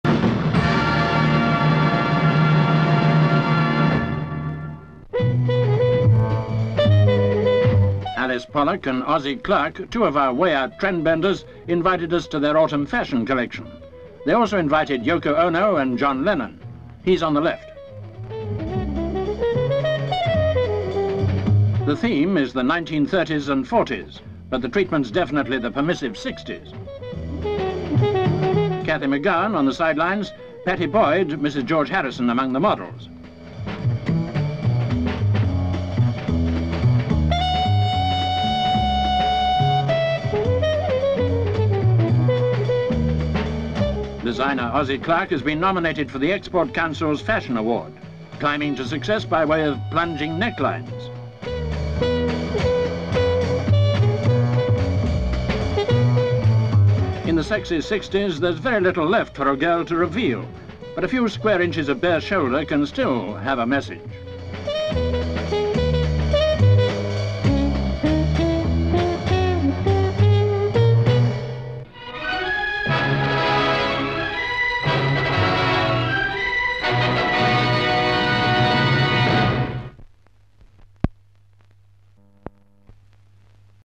"DARING DUO"
(12 Aug 1968) Their names - Alice Pollock and Ossie Clark - two of our most way-out past setters. Their show of Autumn fashions reveals more than a trend. With Beatles among Scene stealers in the audience, the show links the "Glamorous Thirties" - with the "Sexy Sixties".